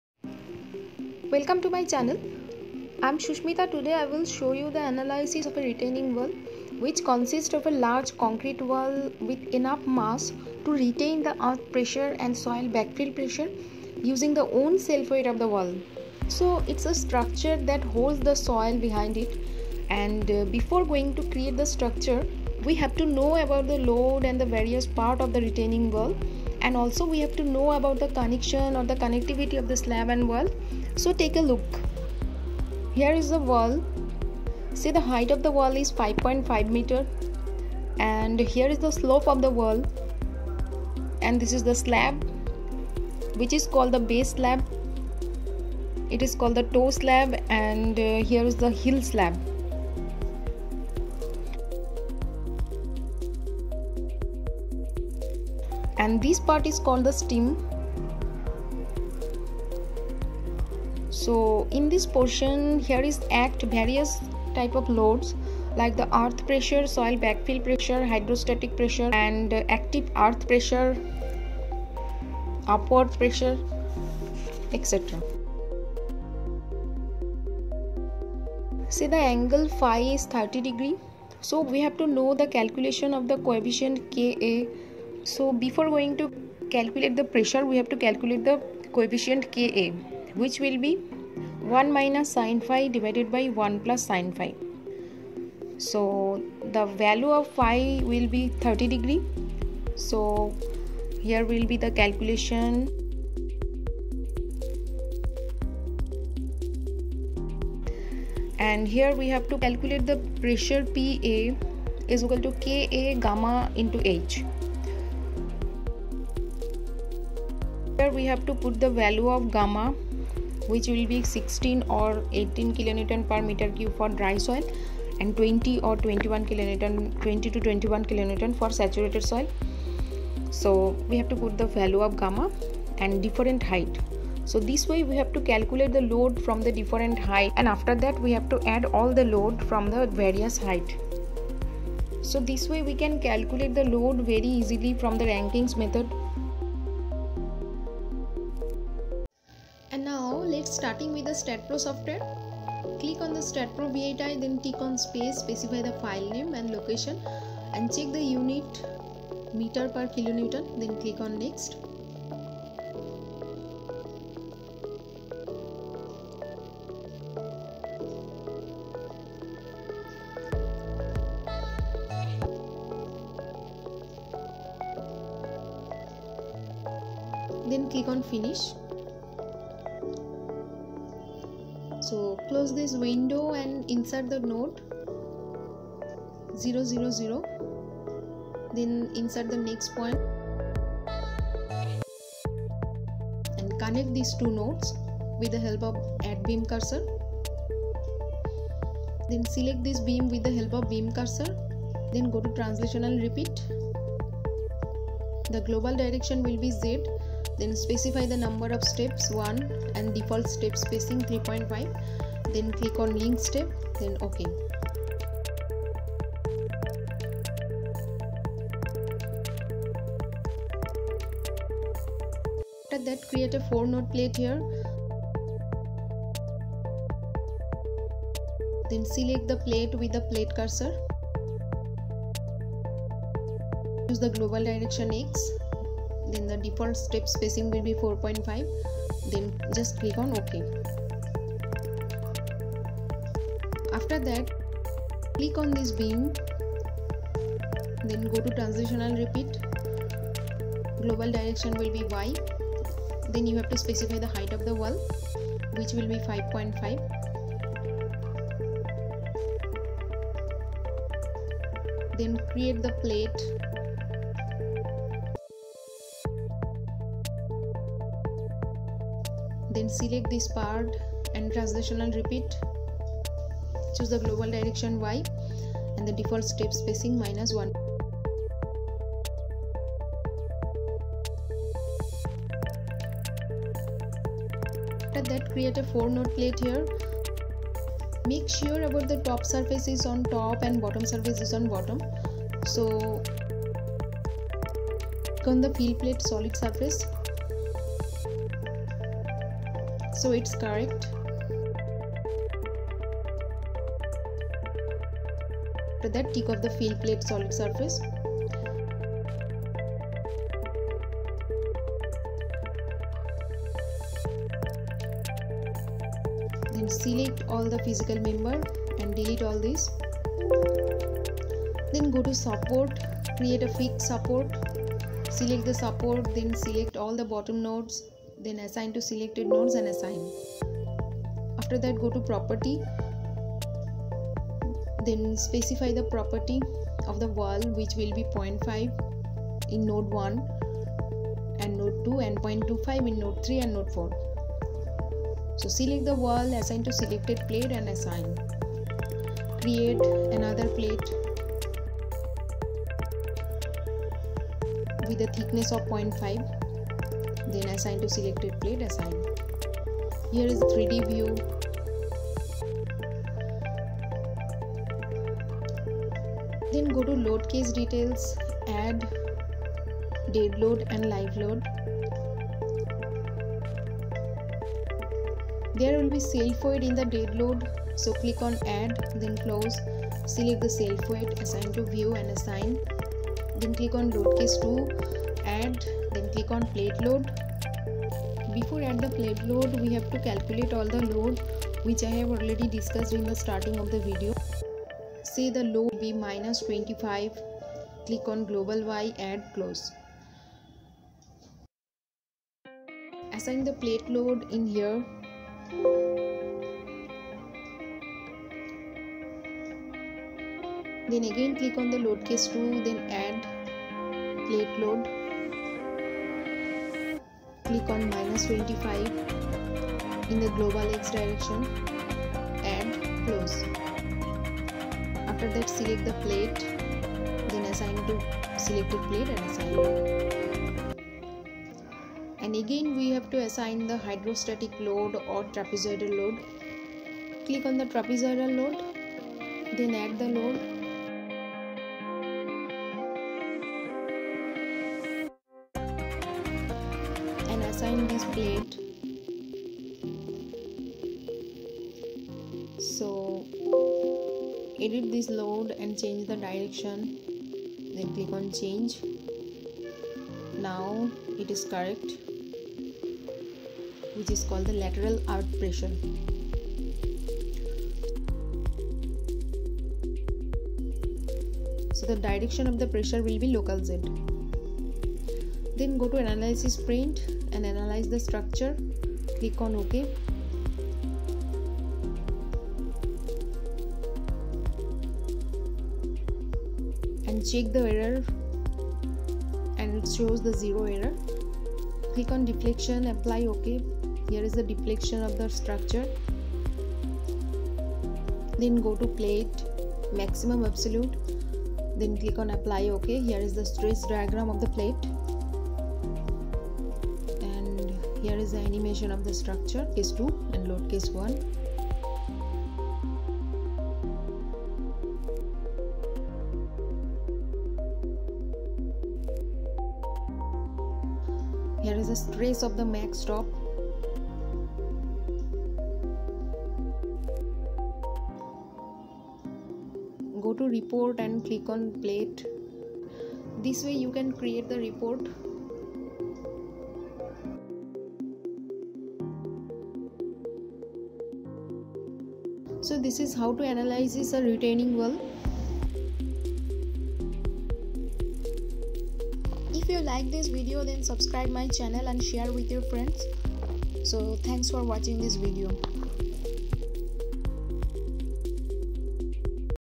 Retaining wall Analysis using by STAAD
ANALYSIS OF RETAINING WALL,Using By Staad Pro V8i.VARIOUS LOAD CALCULATION,LIVE LOAD WITH TRAPEZOIDAL LOAD VALUE ABOVE 20KN LOAD.SOIL BACKFILL PRESSURE,EARTH PRESSURE WITH RANKINE'S THEORY.PLATE STRESS,ANIMATION AFTER APPLYING LIVE LOAD,